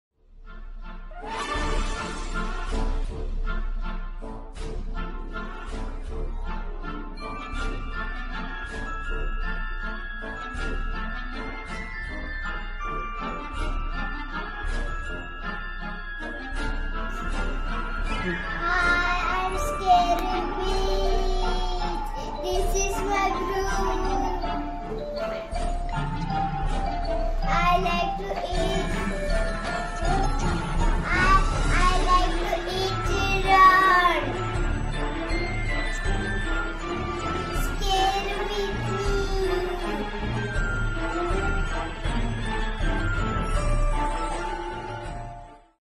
scary witch halloween costume with garbage bag and chart paper..☠☠☠👹👺
scary hallowen witch costume from garbage bag and chart paper.